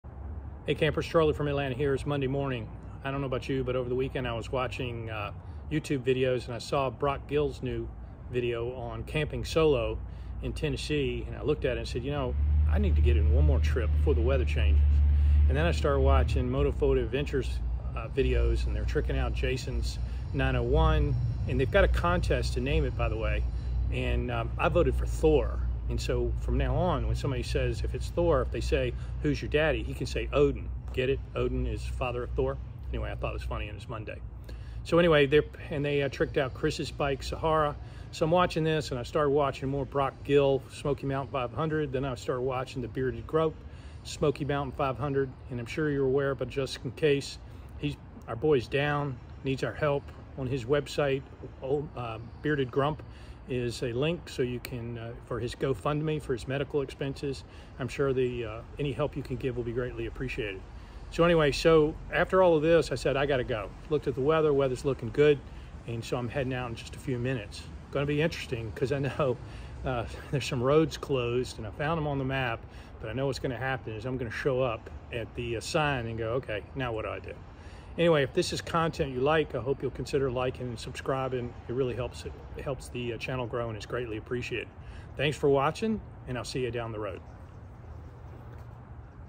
SM500 announcement
I am headed out on the SM500 today, 13 November, due to inspiration I got from Brock Gill, MotoPhoto Adventures and The Bearded Grump. This is the kickoff video